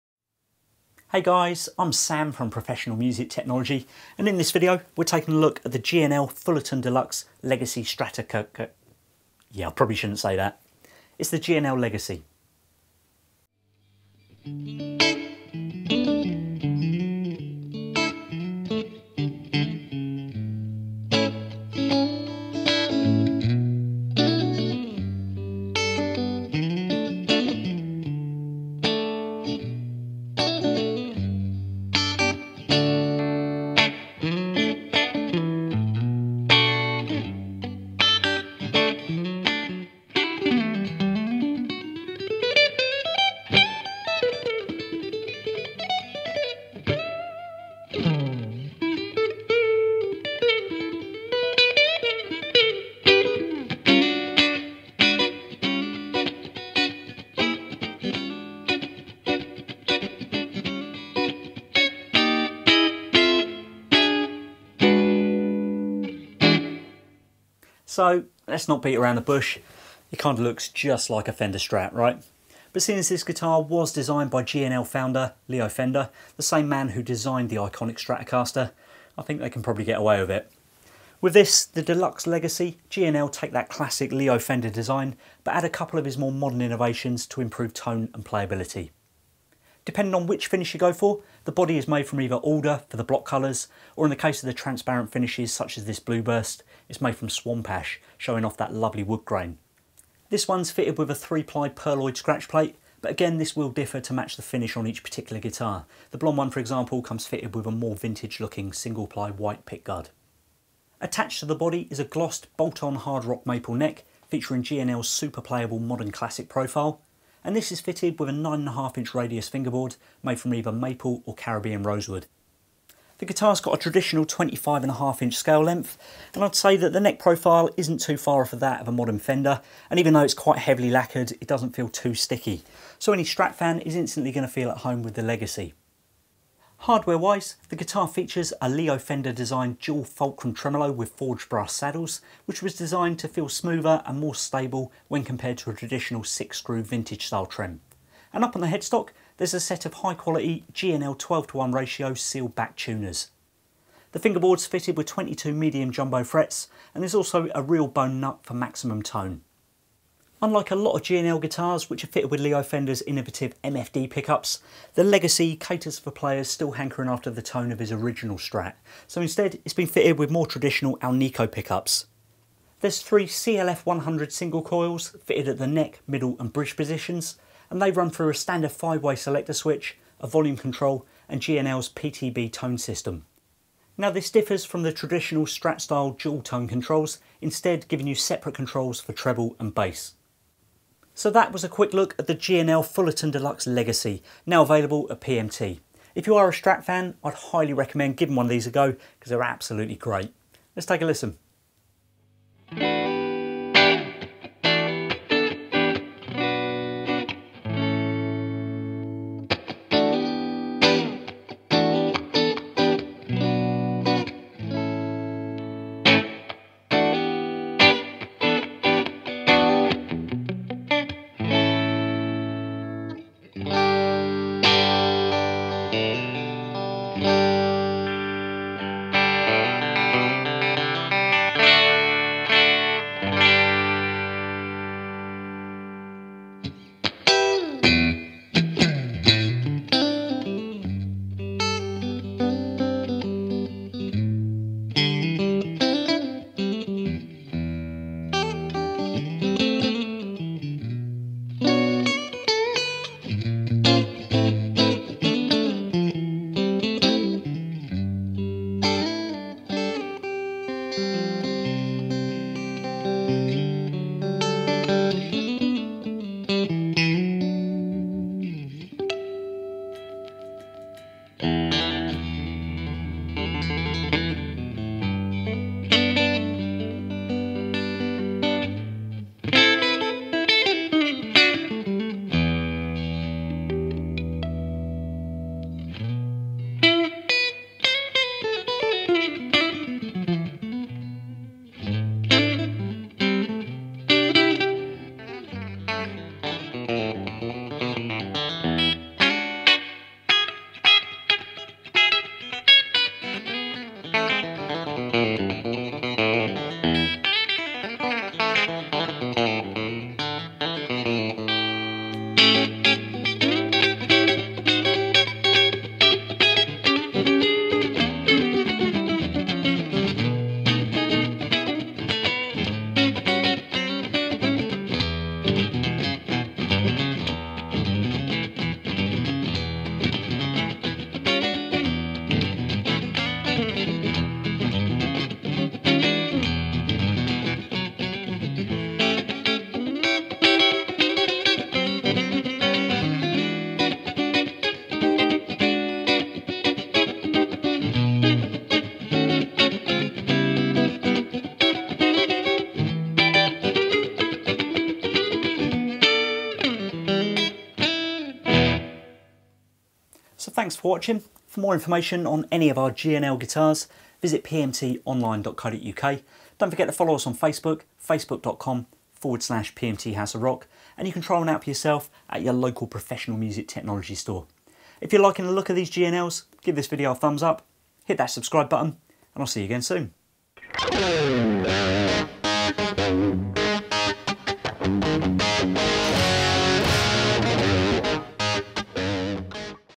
G&L Fullerton Deluxe Legacy - Review & Demo
In this video, Sam takes a look a the G&L Fullerton Deluxe Legacy, an awesome alternative to the Fender American Stratocaster.

So let’s not beat about the bush, the Legacy kinda looks exactly like a Fender Strat, right? But seeing as this guitar was designed by G&L founder Leo Fender, the same man who designed the iconic Stratocaster, we think they can probably get away it!  With this, the deluxe legacy, G&L take that classic Leo Fender design, but add a couple of his more modern innovations to improve tone and playability.

Depending on which finish you go for, the body is made from either Alder for the block colours, or in the case of the transparent finishes such as this blue burst, it’s made from Swamp Ash, showing off that lovely wood grain.  This one’s fitted with a 3 ply pearloid scratch plate, but again this will differ to match the finish on each particular guitar, the blonde for example comes fitted with a more vintage looking single ply white pick guard.  Attached to the body is a glossed, bolt-on hard rock maple neck featuring G&L’s super playable Modern classic profile, and this is fitted with 9.5” radius fingerboard made from either a maple or caribbean rosewood fingerboard.   The guitar’s got a traditional 25.5” scale length, and we’d say that the neck profile’s not too far off of that of a modern Fender, and even though it’s quite heavily lacquered, it doesn’t feel too sticky, so any Strat fan is instantly gonna feel at home with the Fullerton Deluxe.

Hardware-wise, the guitar features a Leo Fender designed Dual Fulcram tremolo with forged brass saddles, which was designed to feel smoother and more stable when compared to a traditional 6 screw vintage style trem, and up on the headstock there’s a set of high quality G&L 12 to 1 ratio, sealed back tuners.  The fingerboard's fitted with 22 medium jumbo frets and there’s also a real bone nut for maximum tone.

Unlike a lot of G&L guitars that are fitted with Leo Fender’s innovative MFD pickups, the Legacy caters for players still hankering after the tone of his original Strat, so instead, it’s been fitted with more traditional alnico pickups.  There’s 3 CLF-100 single coils fitted at the neck, middle and bridge positions and they run through a standard 5-way selector switch, a volume control and G&L’s PTB tone system.  Now this differs from the traditional Strat style dual tone controls which, instead giving you separate controls for Treble and Bass.

Take a quick look and listen with PMTVUK and try one out at your local Professional Music Technology...